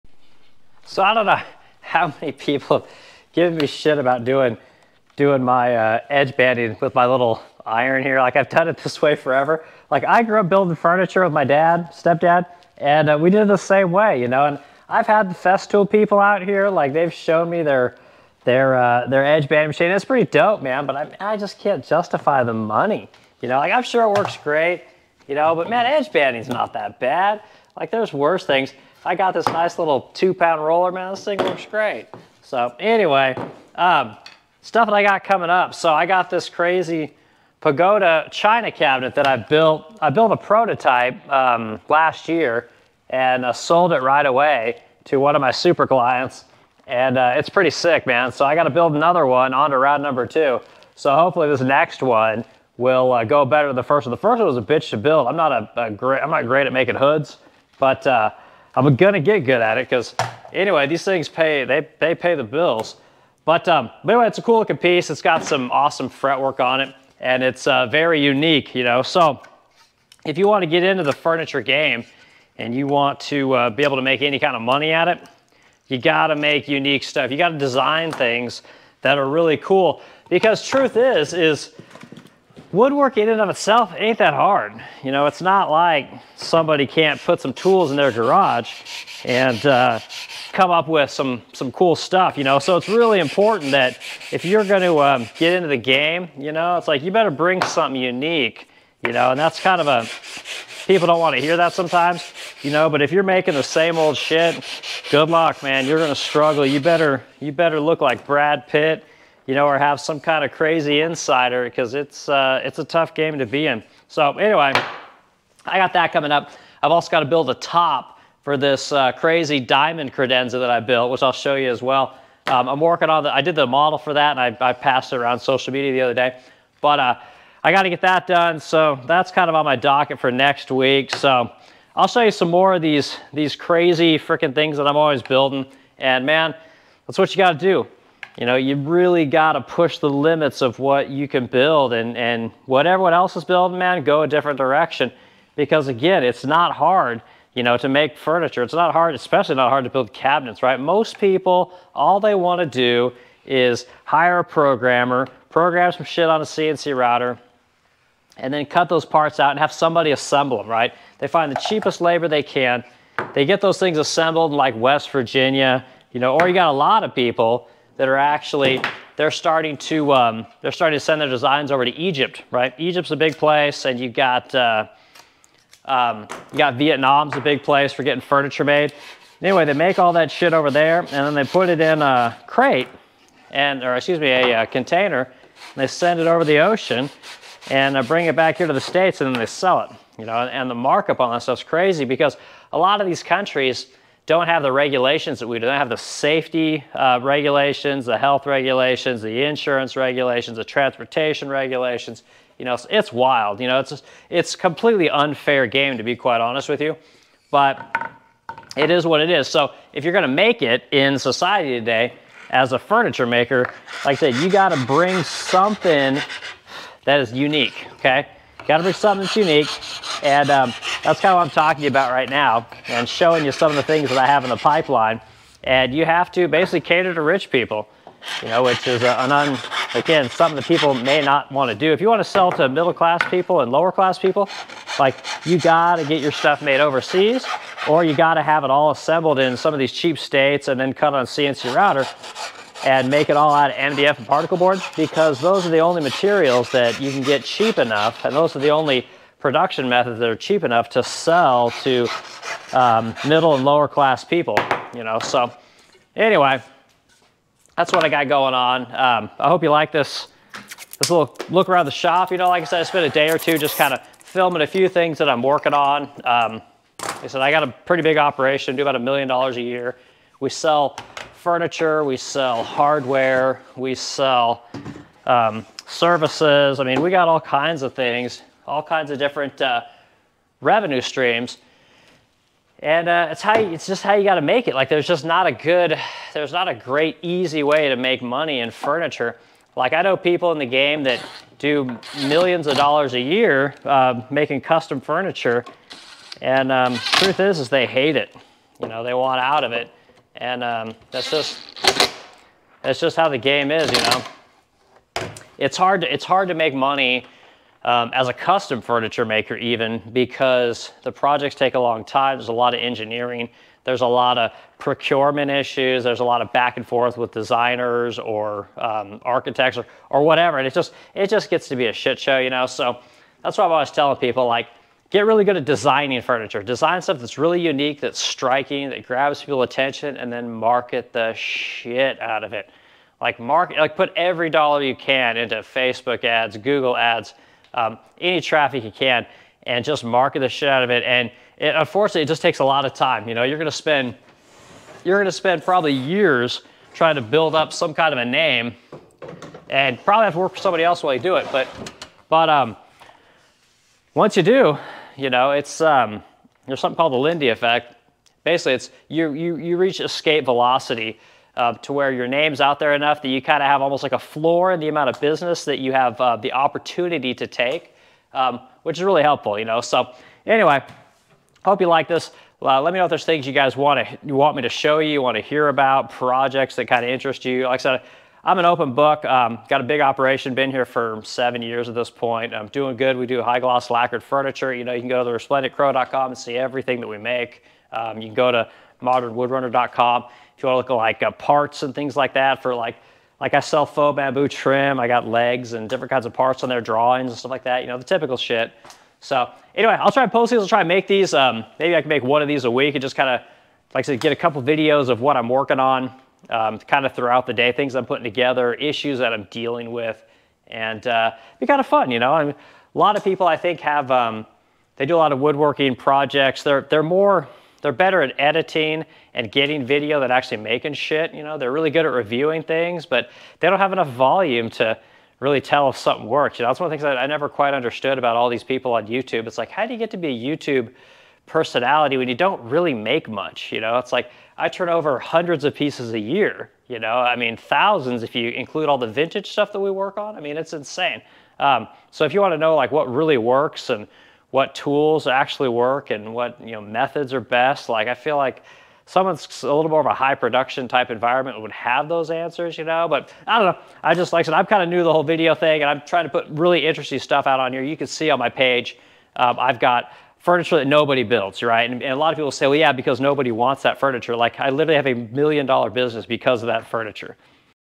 Talking Shop w/ Modern Wood Runner - Making It In The Furniture Business, Design, Rich Customers
New Series of conversations about all things furniture and cabinet making business.  I'll try and talk weekly about what's going on at The Resplendent Crow, client issues, industry BS and how the hell I make it as a professional woodworker in America today